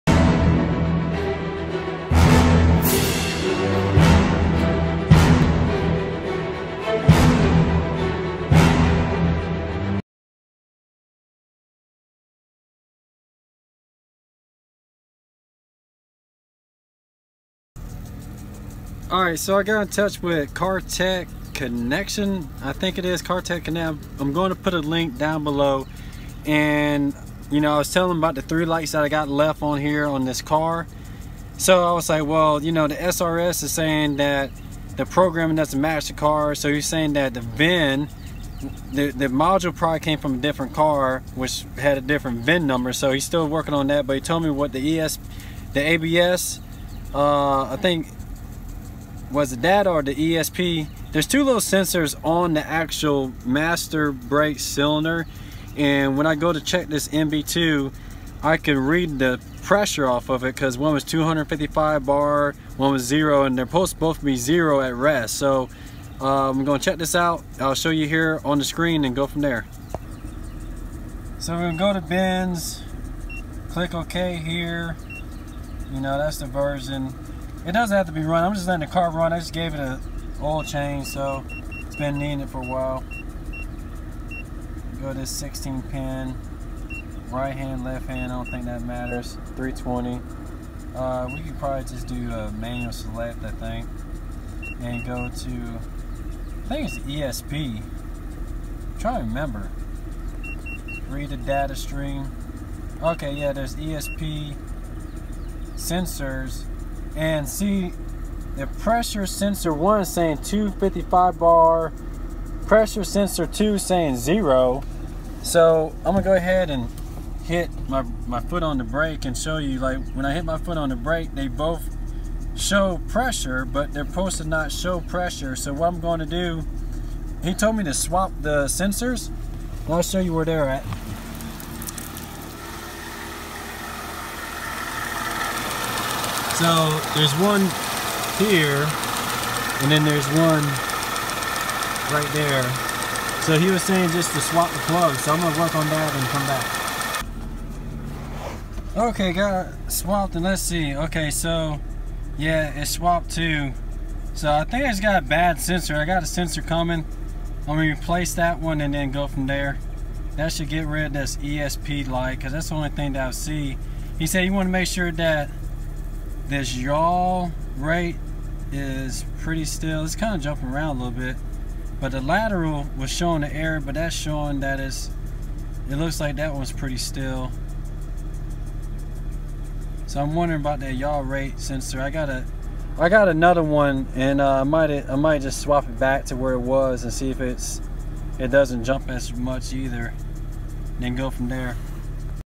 RESTORING 2004 SLK 320 - PART 28 - BENZ ESP PRESSURE SENSOR TROUBLESHOOTING
In no way is this an exact how to and I’m not a certified mechanic. Work on you own vehicle at your own risk. Please be careful and double check bolt and nut to make sure you are installing parts properly.

In this episode I discuss how to test the ESP pressure sensor that is located on the master brake cylinder

This is part 28 of the 2004 slk 320 restoration

If you have a carsoft mbii scanner, or star diagnostics then you can read the esp pressure sensor.  On the r170 platform there are 2 sensors.  At rest the pressure sensors should read 0bar.  Then when you press on the brake pedal inside the car the readings should go up according.  I noticed that on this particular car, that sensor 1 was reading 255 bar at rest while sensor 2 showed 0 bar.  When i pressed on the brake pedal both readings went up to approx 25 bar.  Although sensor 1 went back to 255 bar at rest and sensor 2 went to 0 bar.  So the proper way to troubleshot is to unplug both sensors and swap the connectors so see if the readings switch and they did.  So when switched sensor 2 according to the scanner read 255 bar and sensor 1 read 0 bar according to the scanner.  So the wiring in the car is working properly.  The esp pressure sensor 1 needs replacing.

ESP Pressure Sensor